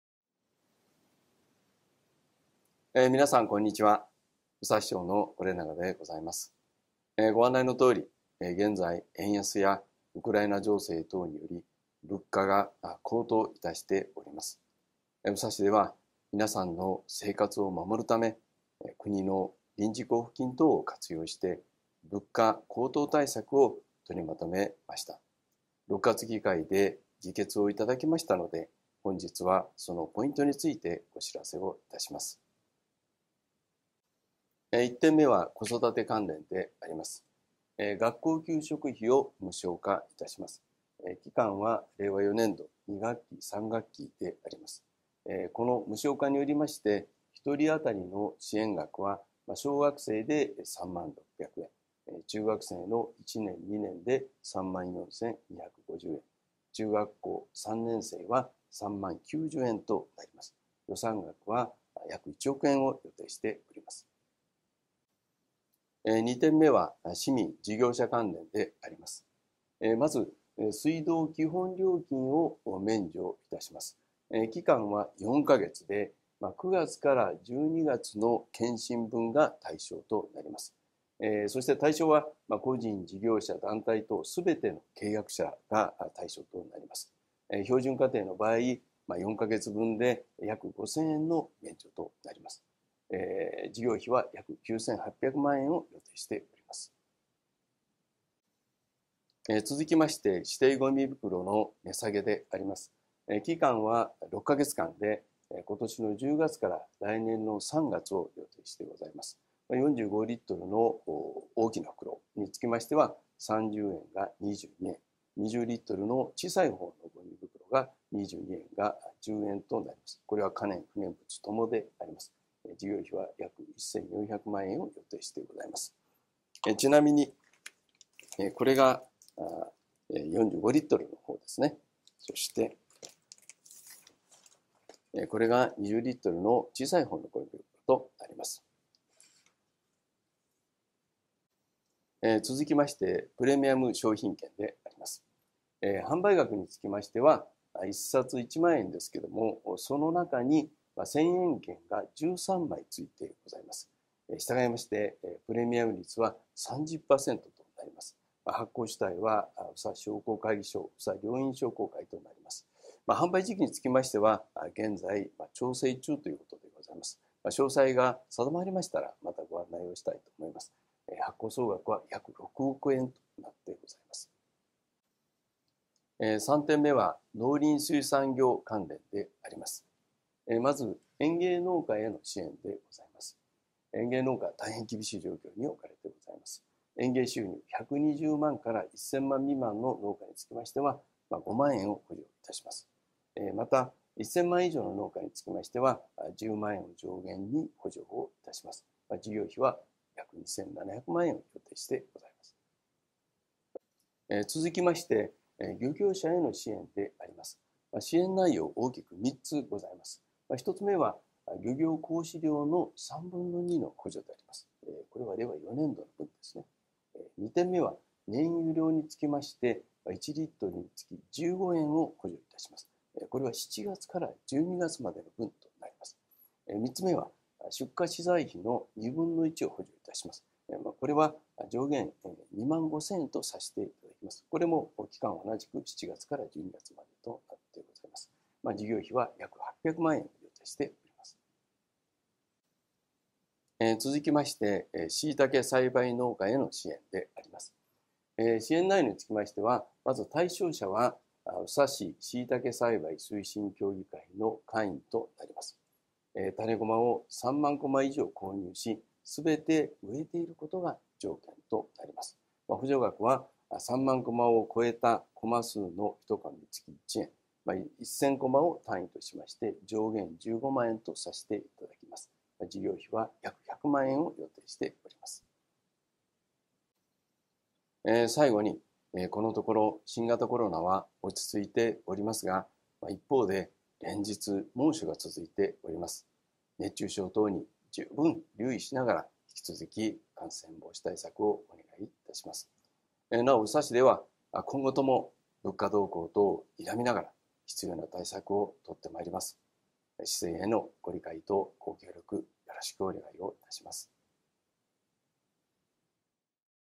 物価高騰対策のポイント
ご案内のとおり、現在、円安やウクライナ情勢等により、物価が高騰しております。宇佐市では皆さんの生活を守るため、国の臨時交付金等を活用して物価高騰対策を取りまとめました。
６月議会で議決いただきましたので、本日はそのポイントについてお知らせします。